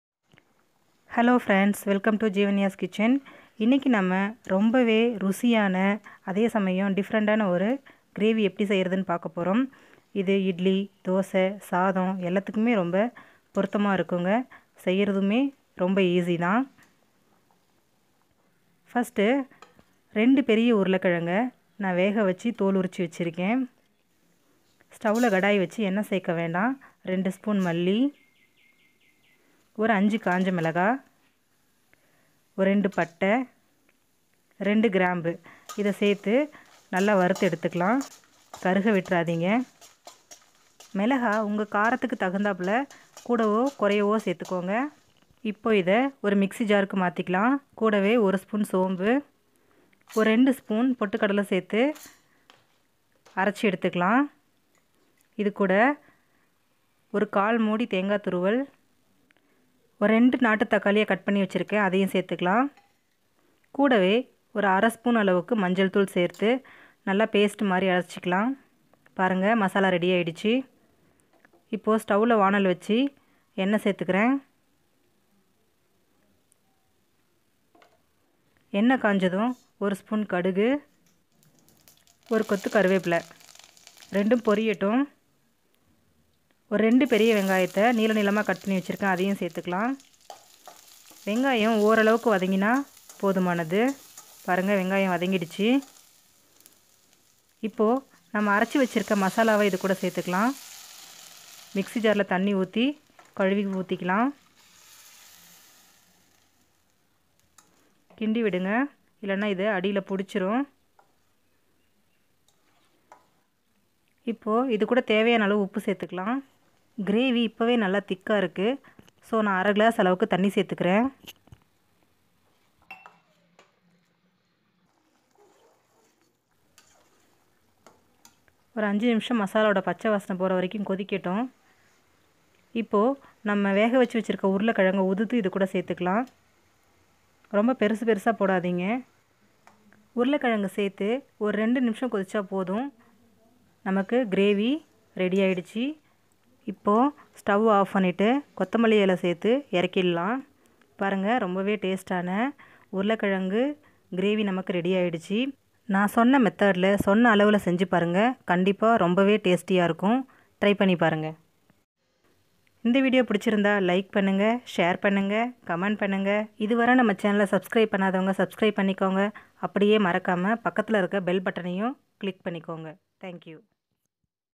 Easy kuruma recipe in Tamil/ உருளைக்கிழங்கு குருமா for idli dosa
In this video we will see how to prepare potato kuruma for idli and dosa. It is very to prepare and very delicious.